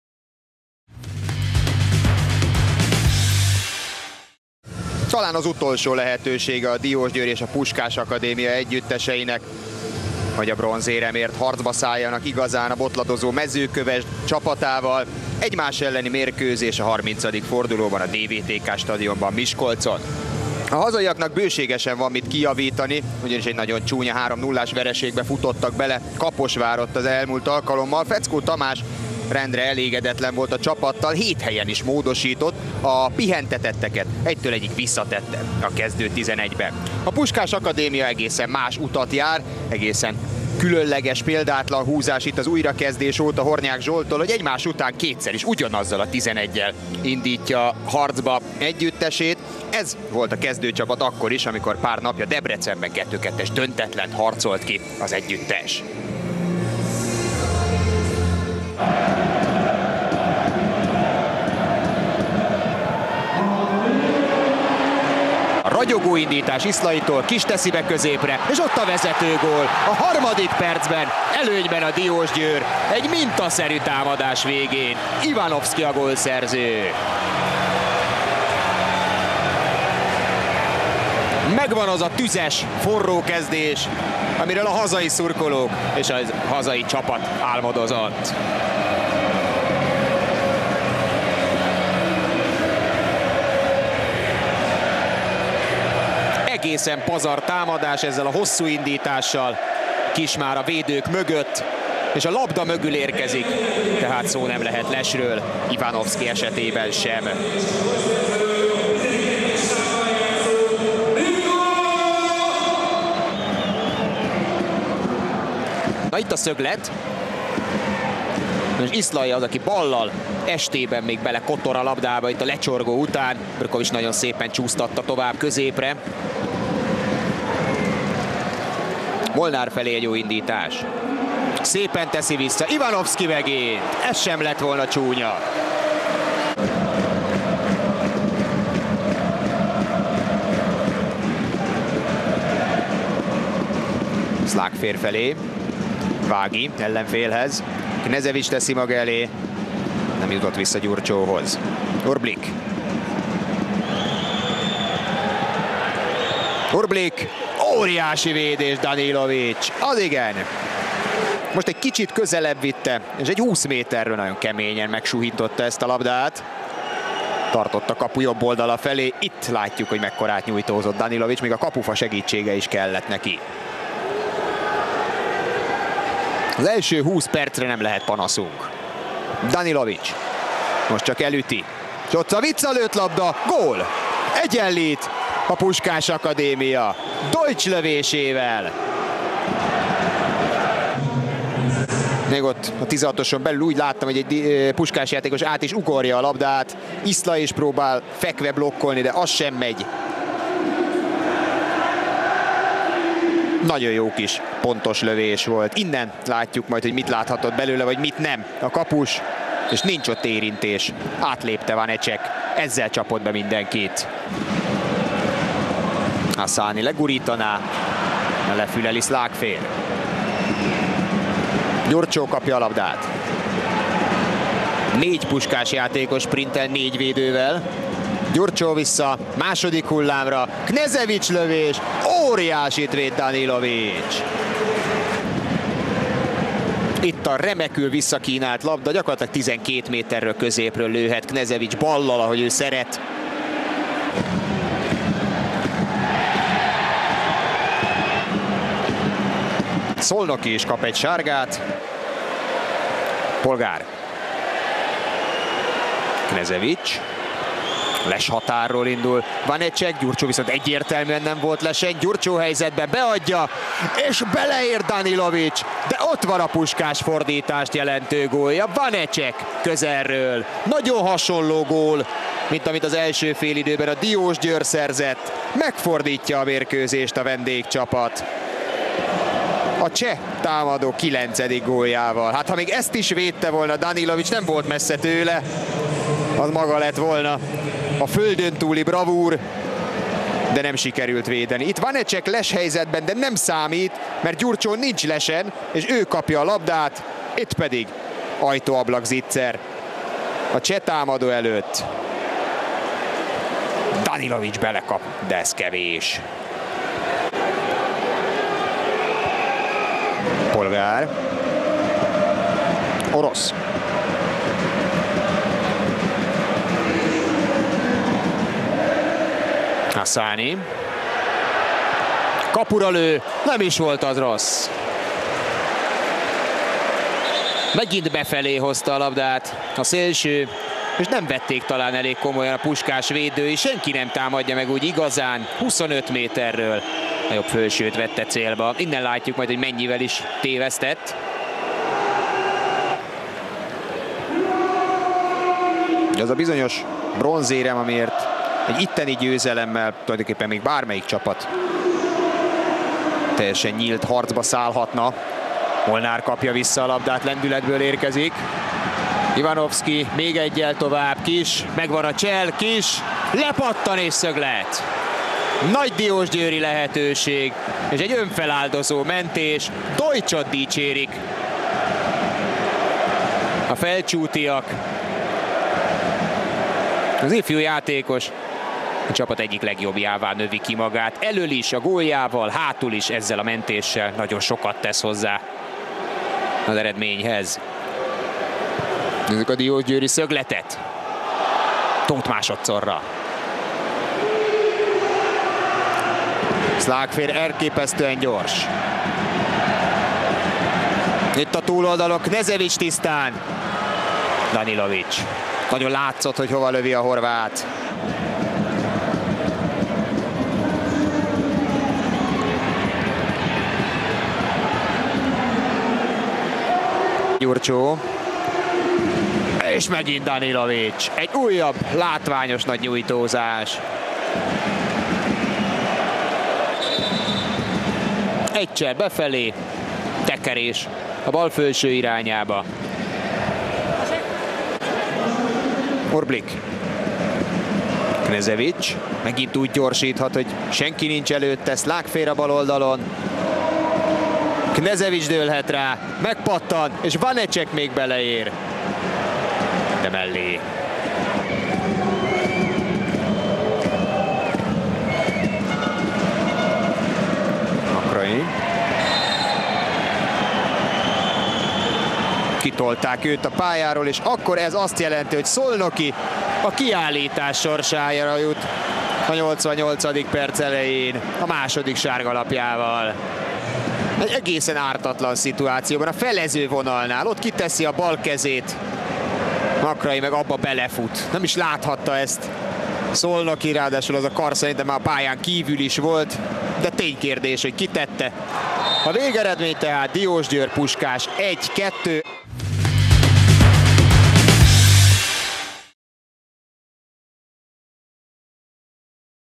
DVTK – Puskás Akadémia FC | 1-2 | OTP Bank Liga | 30. forduló | MLSZTV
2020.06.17. OTP Bank Liga, 30. forduló
DVTK – Puskás Akadémia FC | 1-2 | (1-1) |
összefoglaló